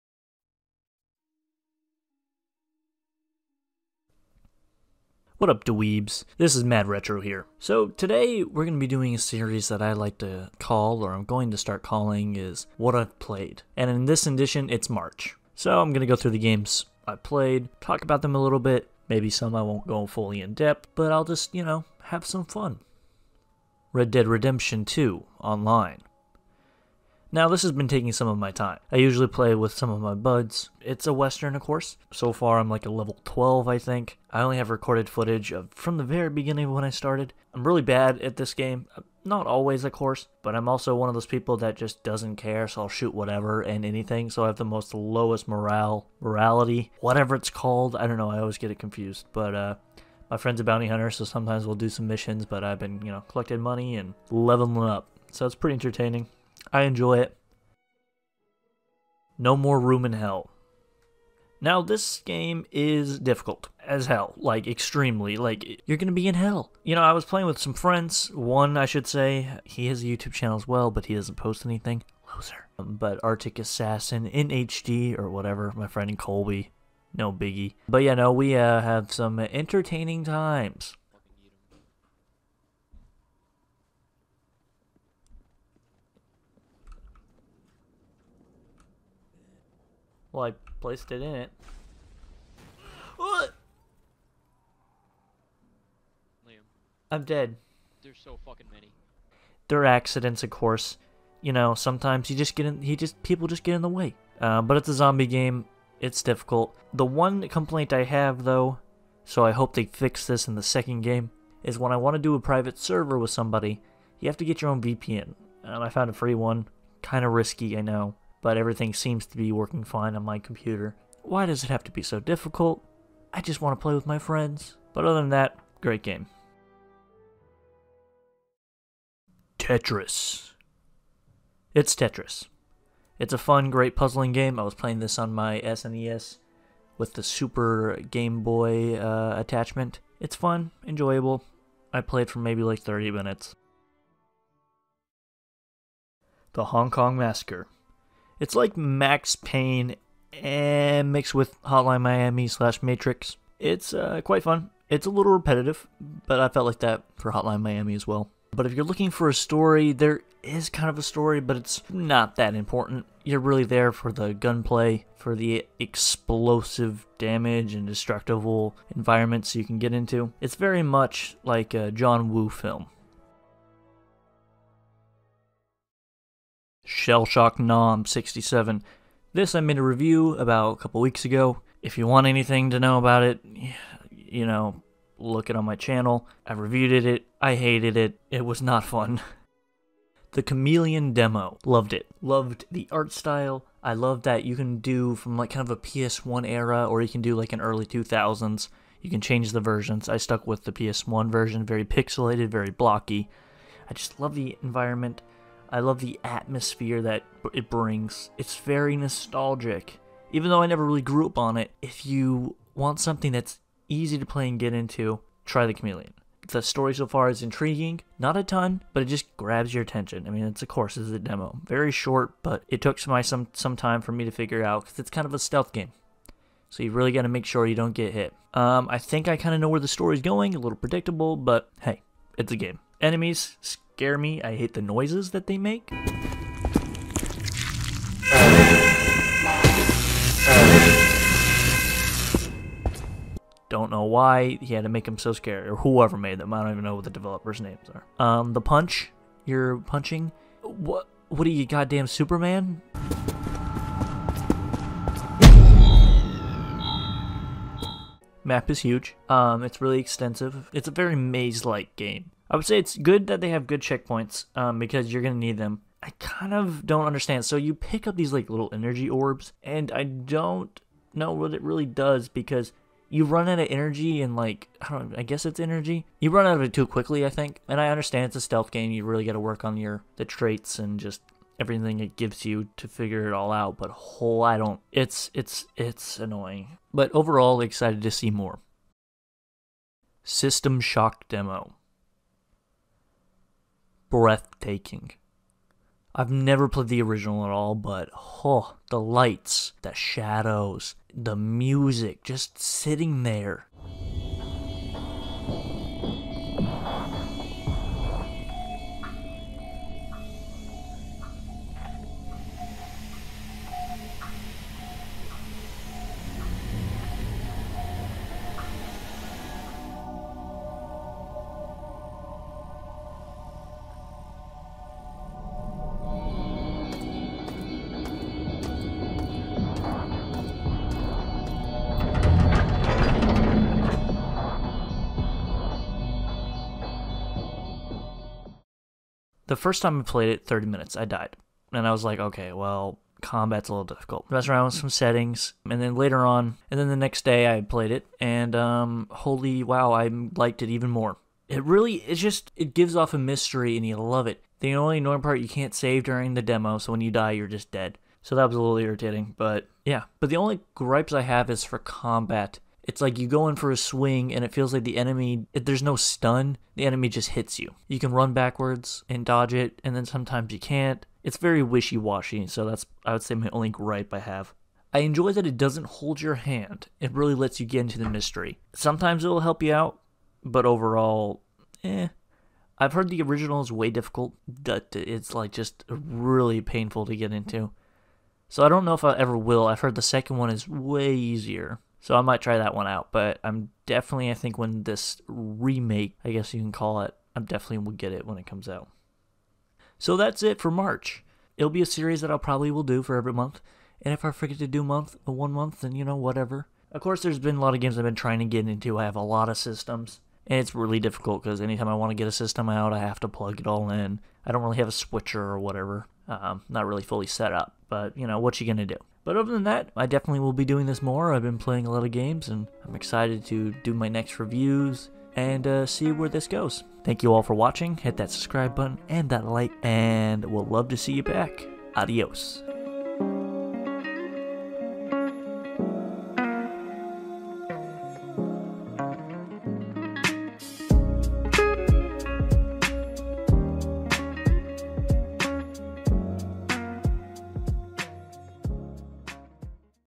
Games I've Played in March
In this video I chat about all the video games I have played in March.
I played games from Tetris to Red Dead Redemption 2 Online. Had an enjoyable time making this non scripted video and will probably make more of these in the future.